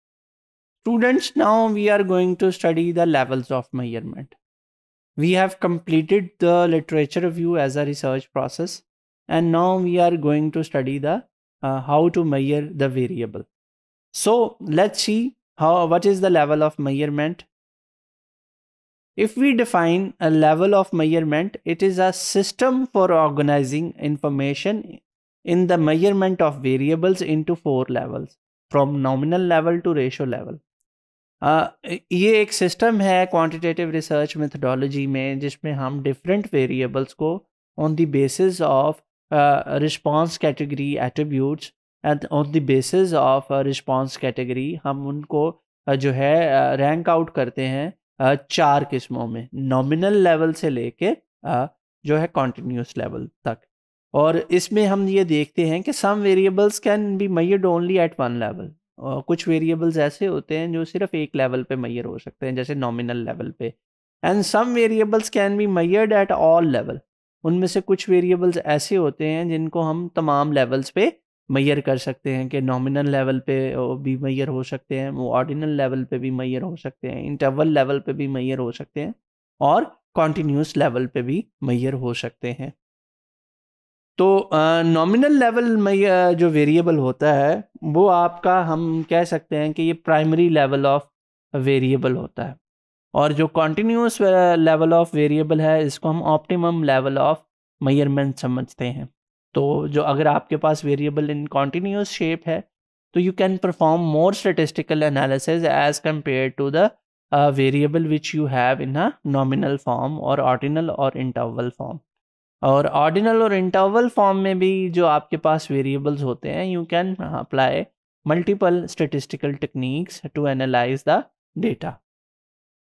Levels Of Measurement | Quantitative Research Methodology | SOC509_Topic047
Topic047:  Levels Of Measurement
By Dr. Qaisar Khalid Mahmood